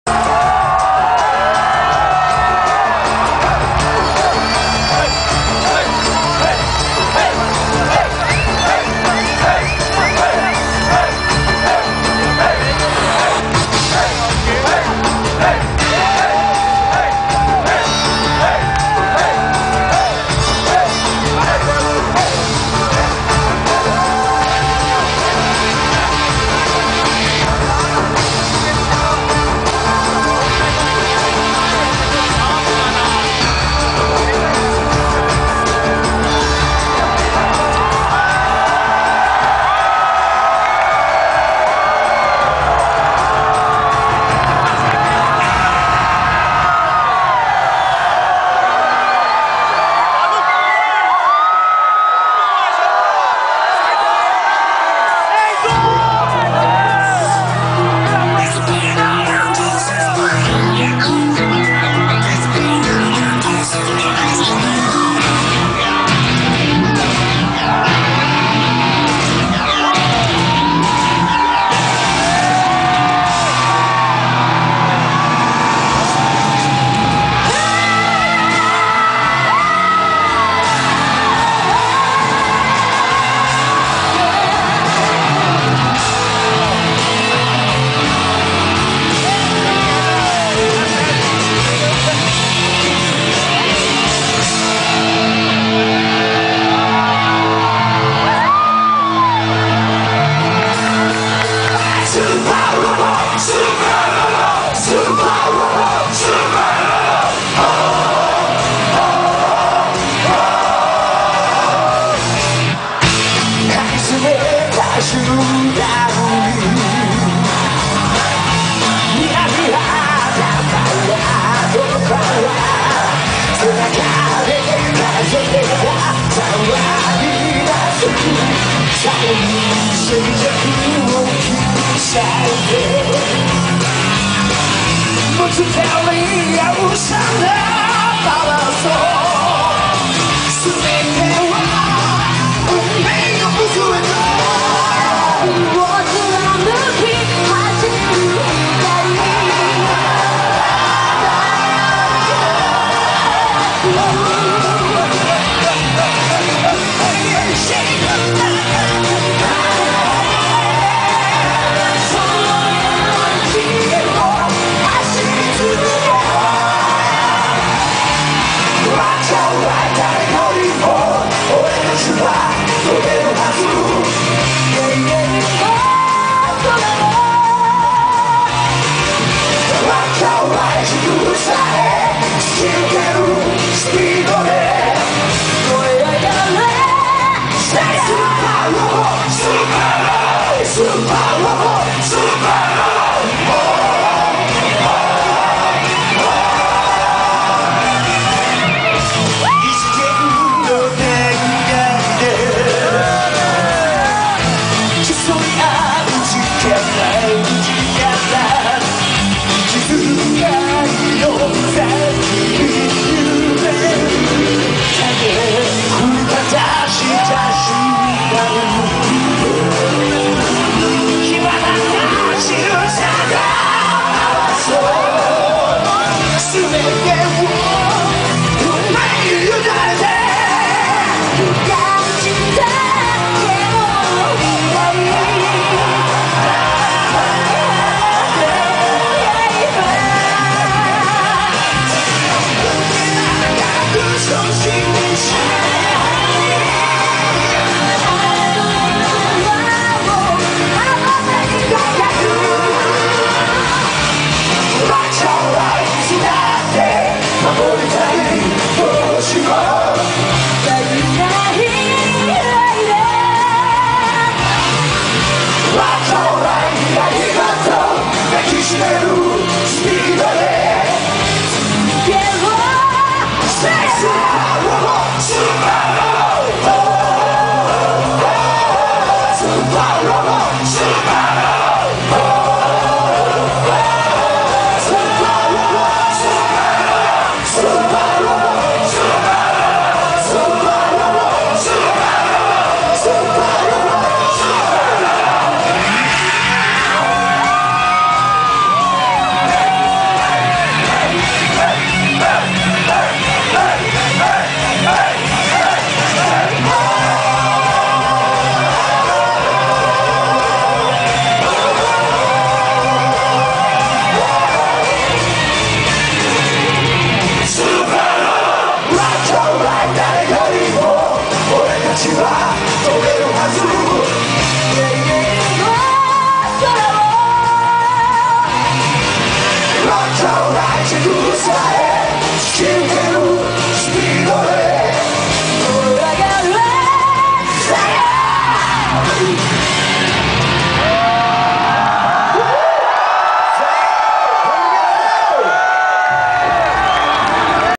Anime Friends 2008 - JAM Project - abertura do show
Abertura do show do JAM Project
Hironobu Kageyama, Masaaki Endo, Kitadani Hiroshi, Yoshiki Fukuyama e Masami Okui
domingo dia 20/07/08